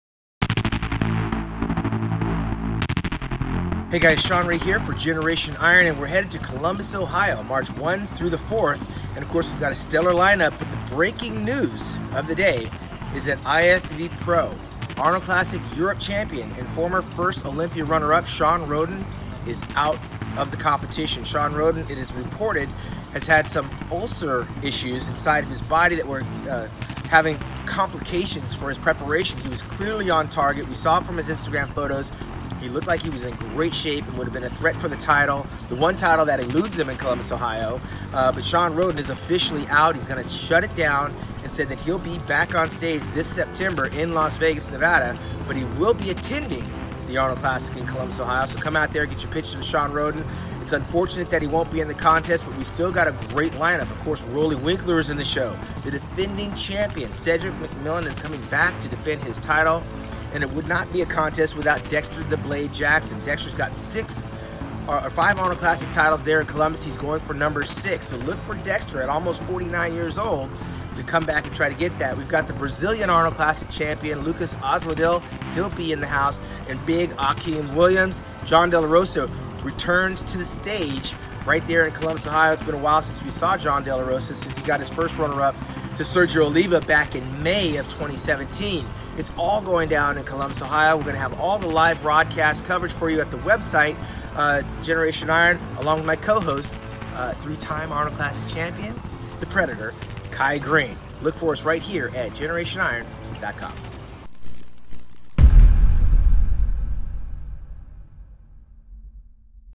Shawn Rhoden Drops Out Of Arnold Classic 2018 | GI News
This past weekend Shawn Rhoden has announced that he will be dropping out of the Arnold Classic 2018 competition due to extreme dehydration caused by illness. Shawn Rhoden was one of many high profile competitors heading into the competition this March for a stellar lineup comparable to the Mr. Olympia competition itself. Shawn Ray gives the full report along with other competition list updates  in our GI news report above!